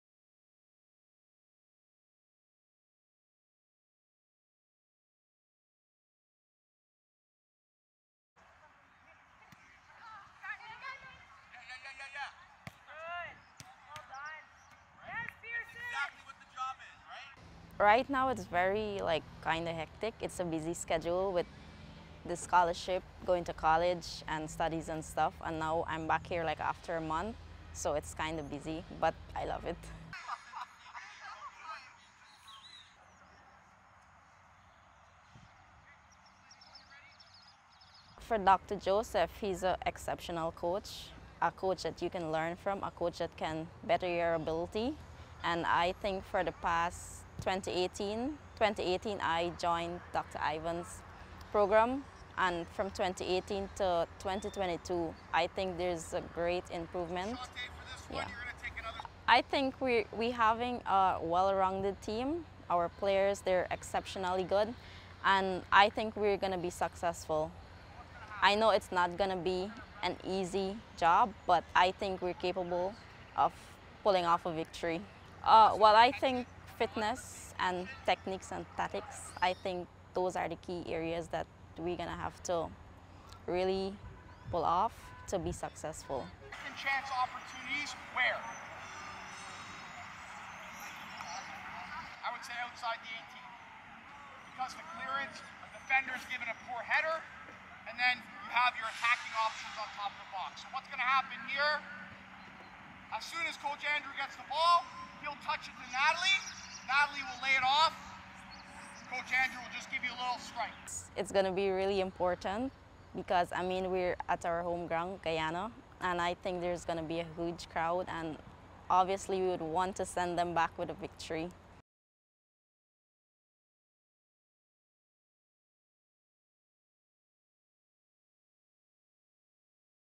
ANNALISA VINCENT BANKING ON HUGE HOME SUPPORT FOR LADY JAGS
Annalisa Vincent, a 22-year-old striker from Moruca in Region One, is calling on Guyana to come out and support the Lady Jags at their World Cup qualifying match against Dominica on Wednesday at Leonora.
Vincent, who recently secured a four-year scholarship to Graceland University in Iowa, United States, said the plan to gift the patrons a win in round one of the qualifiers.
Match time is 20:00h.